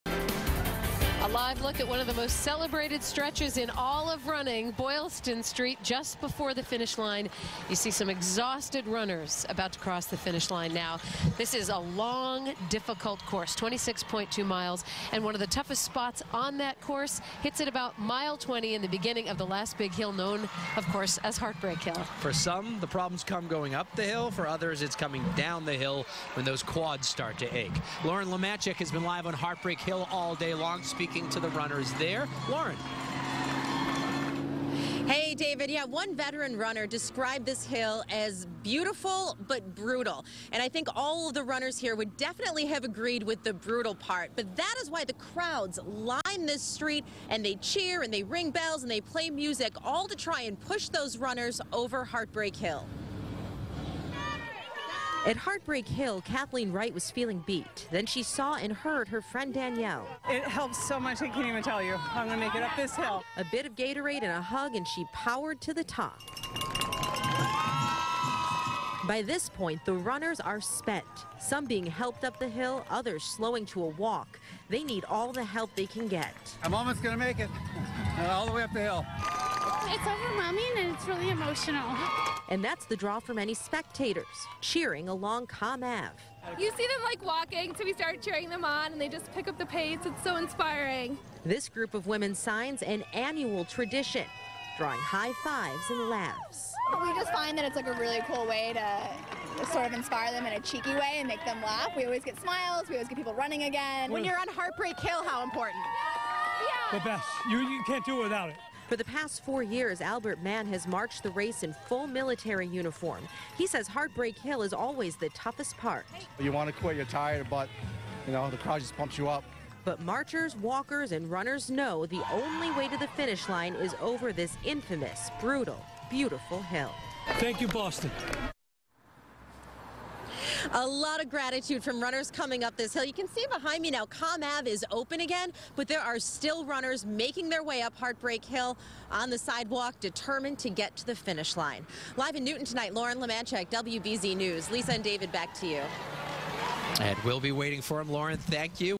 Determined Marathon Runners Take On Heartbreak Hill
Heartbreak Hill in Newton is a big obstacle for Boston Marathon runners. WBZ-TV's Lauren Leamanczyk reports.